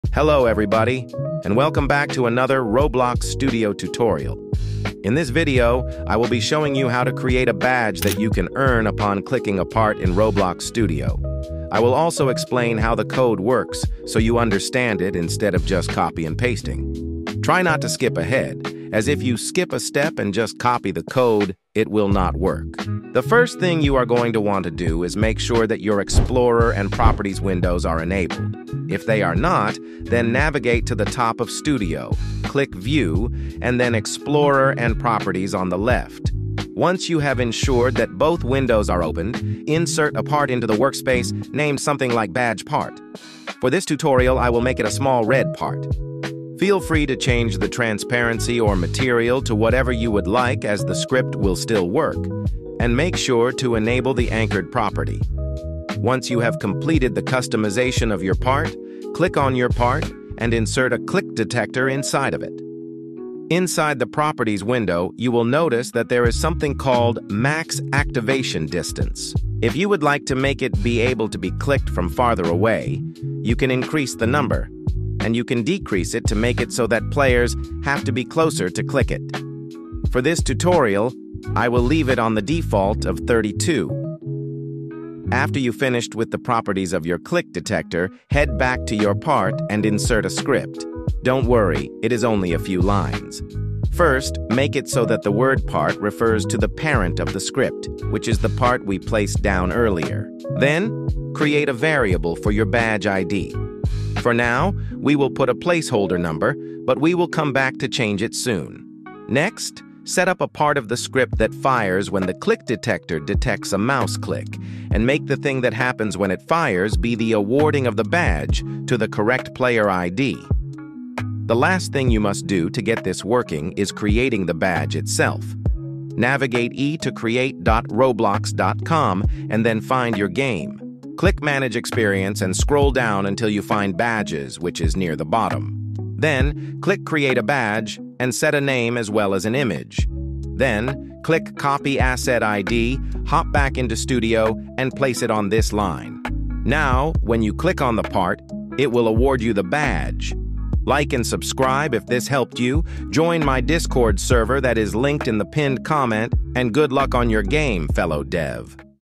How To Award A Badge ON CLICK In Roblox Studio!
In this video, I will show you how to use badges in Roblox studio. It shows how to create a badge, and it also shows how to award the badge. This video shows how to award a badge on click. I have another video on awarding it on touch.

Join the official Discord server to chat with myself and other devs, copy and paste scripts, enter giveaways, and more